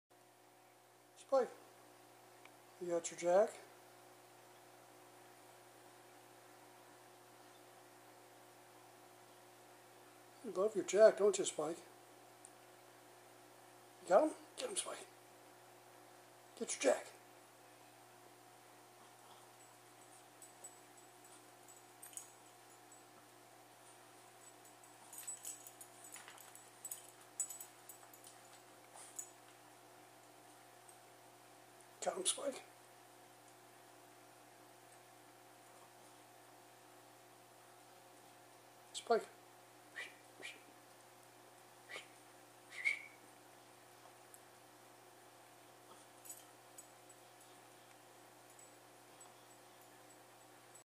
boston terrier with his toy
just thought i would film spike for a minute with his toy jack :)